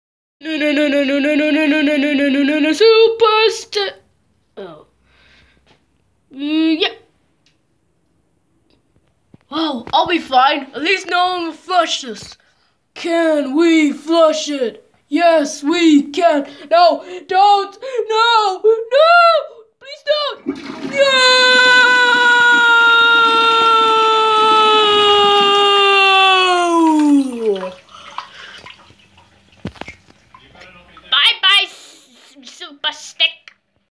Super Stick gets flushed away
Super Stick fails to do parkour and drowns in a whirlpool of dispatch